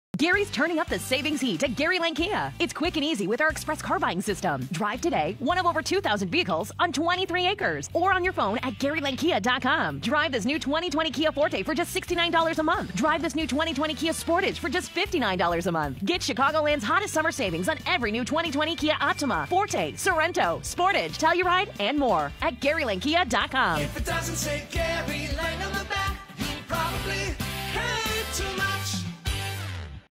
July is your time to buy at Gary Lang Kia!
July is your time to buy and now the savings are hotter than ever! Hurry in while the selection is HOT HOT HOT! We are ready to help you find the perfect car for your budget. And don't forget.. challenged credit, don't sweat it! We'll do everything we can to help you get financed! Visit us today on Route 31 in McHenry and remember...IF IT DOESN'T SAY GARY LANG ON THE BACK, YOU PROBABLY PAID TOO MUCH!